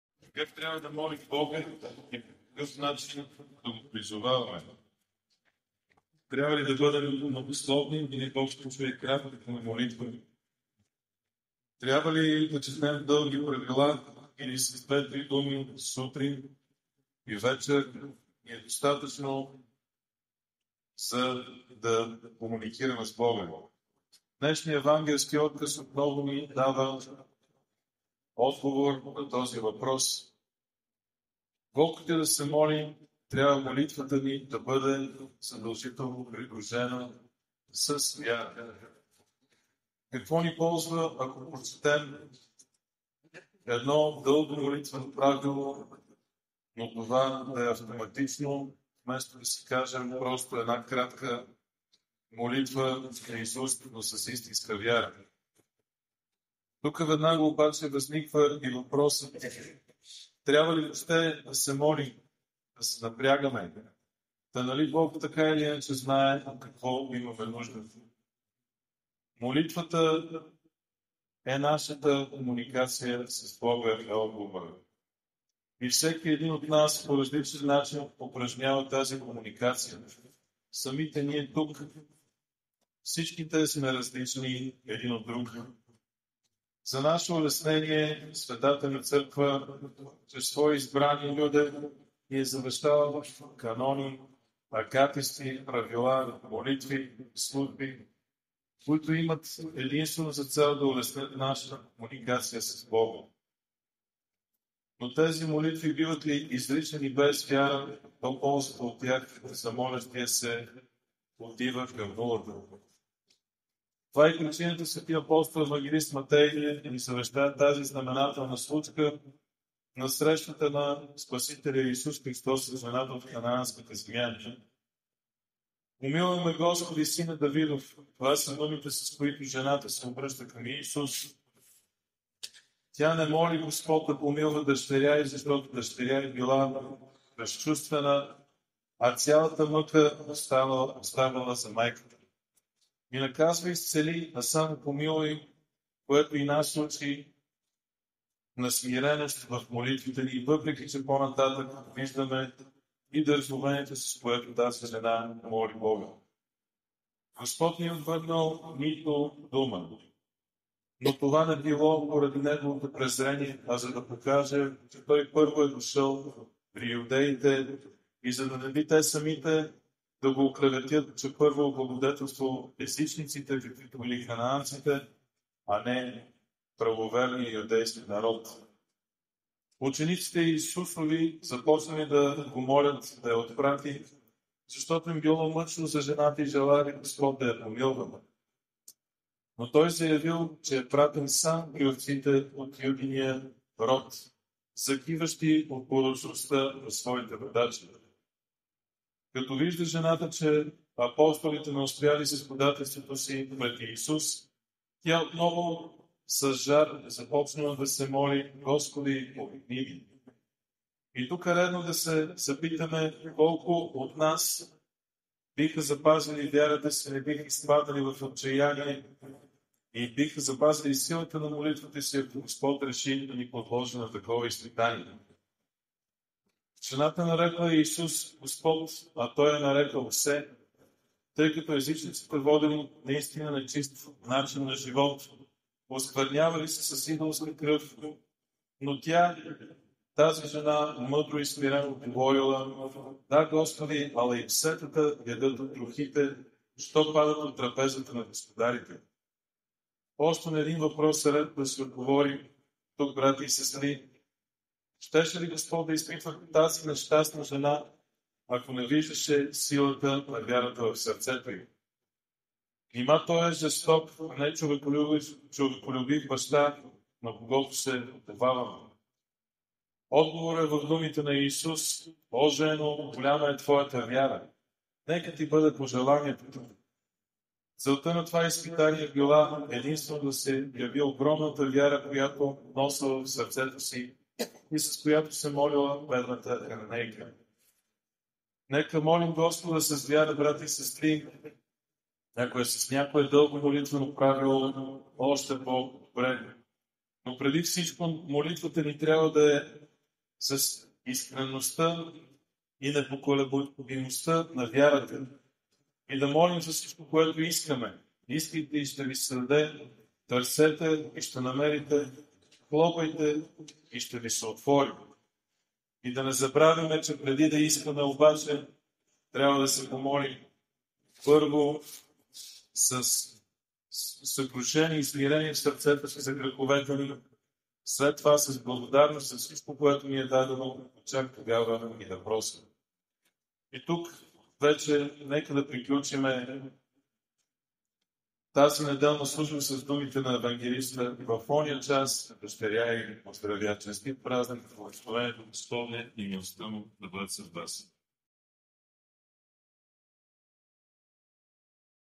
Неделя на хананейката - Проповед на о. Антоний
Проповед на отец Антоний, катедрален храм Св. Александър Невски, София

18 февруари
† Неделя 17 след Петдесетница – на Хананейката. Св. Лъв, папа Римски. Св. Агапит, еп. Синадски.

ВТОРО ПОСЛАНИЕ НА СВЕТИ АПОСТОЛ ПАВЛА ДО КОРИНТЯНИ 6:16-18;7:1
16. Каква прилика между Божия храм и идолите? Защото вие сте храм на живия Бог, както е казал Бог: "ще се поселя в тях и ще ходя между тях; ще им бъда Бог, а те ще бъдат Мой народ".

17. "Затова излезте из средата им и се отделете, казва Господ, и до нечисто се не допирайте, и Аз ще ви приема";

18. "и ще ви бъда Отец, а вие ще бъдете Мои синове и дъщери", казва Господ Вседържител.

1. И тъй, възлюбени, имайки такива обещания, нека се очистим от всяка сквернота на плътта и на духа, като вършим свети дела със страх Божий.

ОТ МАТЕЯ СВЕТО ЕВАНГЕЛИЕ 5:21-28
21. Слушали сте, че бе казано на древните: "не убивай; а който убие, виновен е пред съда".

22. Аз пък ви казвам, че всеки, който се гневи на брата си без причина, виновен ще бъде пред съда, а който каже на брата си: "рака", виновен ще бъде пред синедриона; а който пък каже: "безумнико", виновен ще бъде за геената огнена.

23. И тъй, ако принасяш дара си на жертвеника, и там си спомниш, че брат ти има нещо против тебе,

24. остави дара си там пред жертвеника и иди първом се помири с брата си, и тогава дойди и принеси дара си.

25. Помирявай се с противника си скоро, докле си още на път с него към съдилището, за да не би някак противникът ти да те предаде на съдията, а съдията да те предаде на слугата и да бъдеш хвърлен в тъмница;

26. истина ти казвам: няма да излезеш оттам, докле не върнеш и последния кодрант.

27. Слушали сте, че бе казано на древните: "не прелюбодействувай".

28. Аз пък ви казвам, че всеки, който поглежда на жена с пожелание, вече е прелюбодействувал с нея в сърцето си.